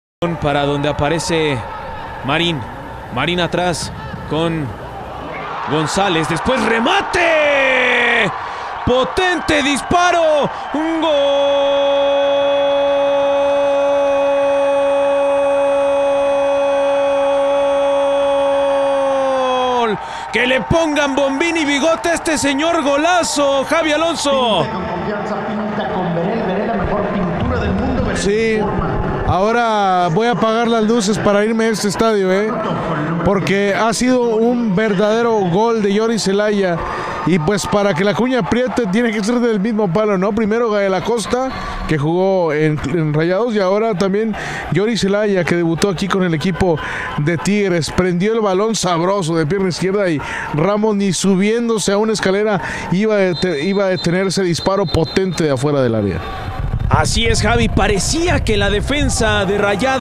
Gol de J. Celaya | Raya2 0-2 Celaya | Jornada 10 - Apertura 2022 - Liga BBVA Expansión MX
¡G⚽⚽⚽⚽LAZ⚽ de Club Celaya! ⚽🔥

¡ESPECTACULAR!

De larga distancia, Jhory Celaya sacó este potente disparo que ponía el 0-2 en el BBVA.